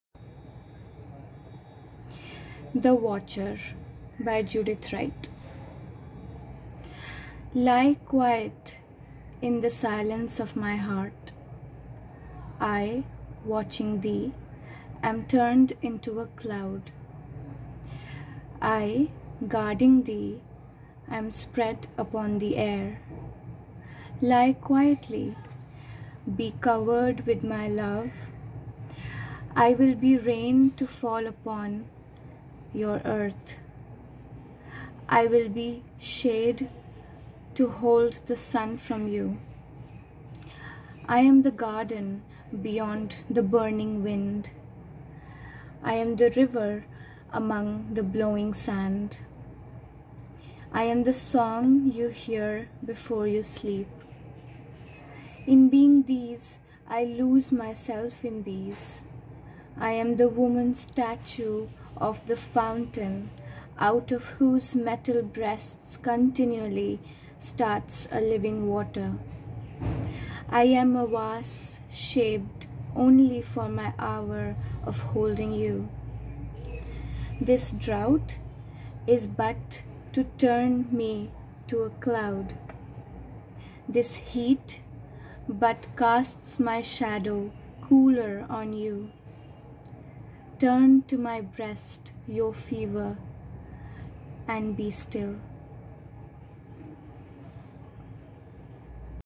"The Watcher" by Judith Wright
Reciting "The Watcher" by Judith Wright.

Ciao.